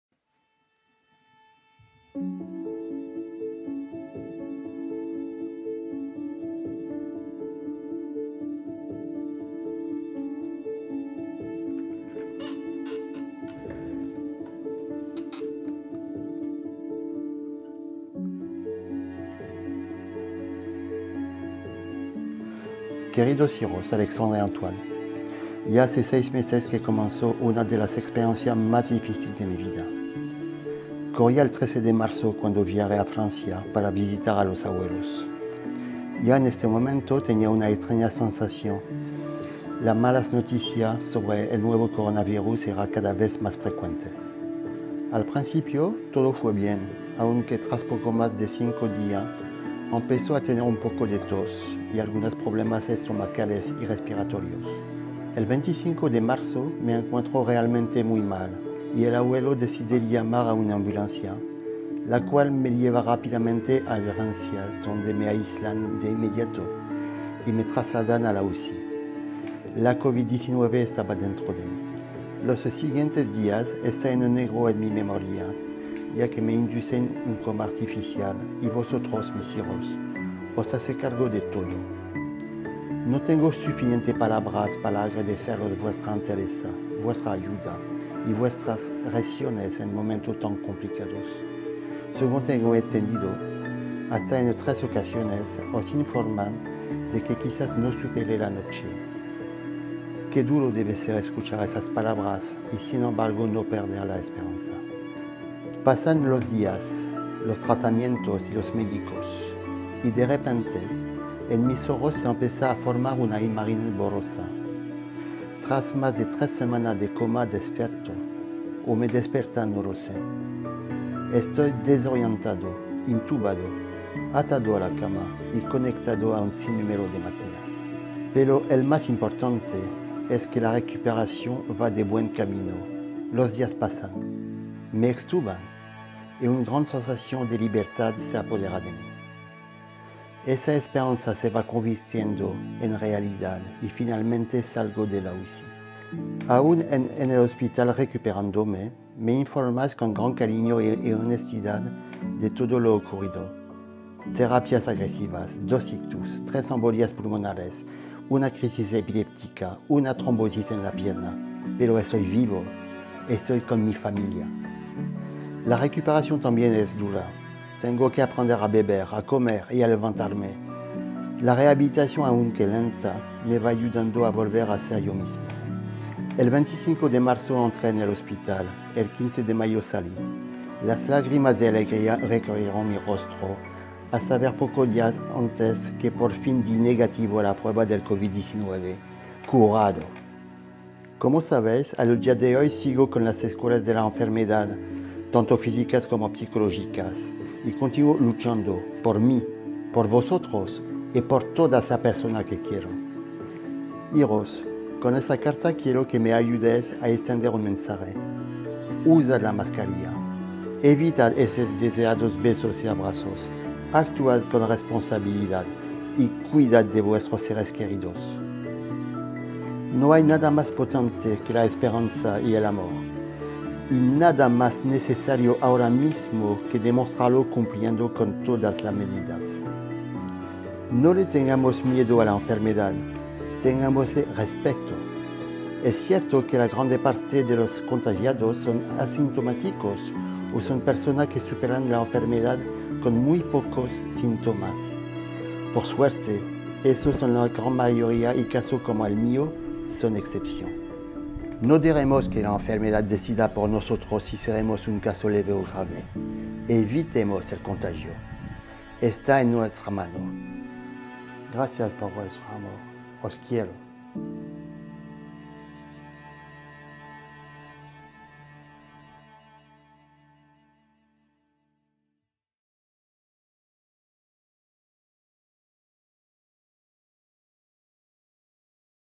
Resistencia y Esperanza. Una carta de Bruno Fievet
Bruno Fievet, marido, padre de dos hijos y compañero de Lopesan Villa del Conde, se está recuperando en estas semanas de las importantes secuelas que le ha dejado la COVID-19. Te contamos su historia. Comenzó el pasado marzo...